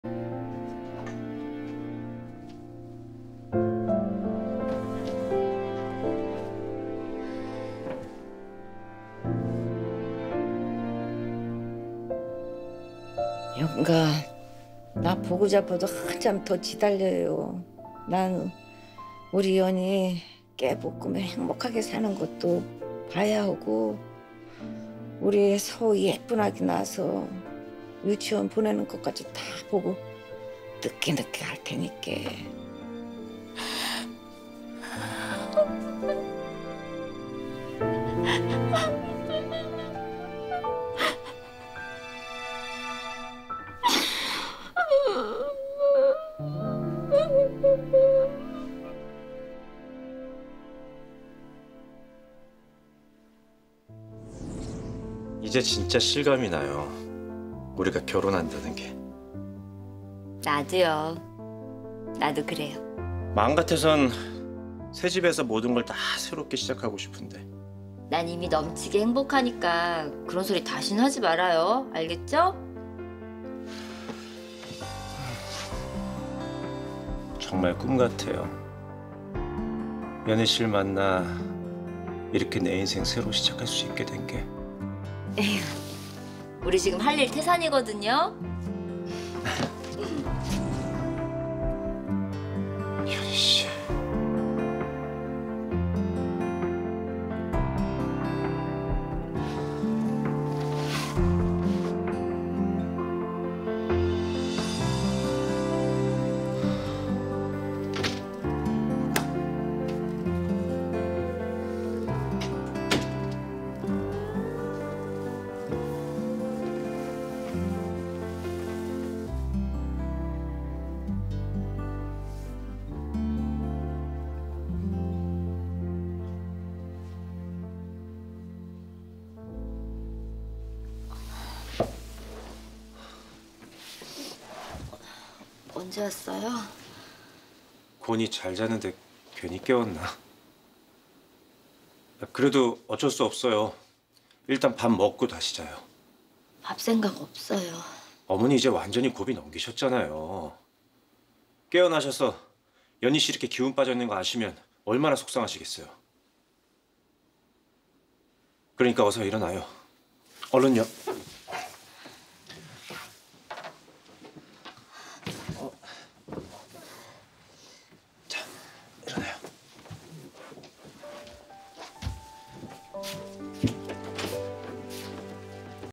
최정윤, 오래 살겠다던 이경진 떠올리며 ‘폭풍 오열’
집으로 돌아온 최정윤(도연희)은 이경진(서순분)을 떠올리며 눈물을 흘린다. 한편 최정윤이 걱정된 안재모(한재경)는 최정윤의 집에 찾아가 최정윤을 위로한다.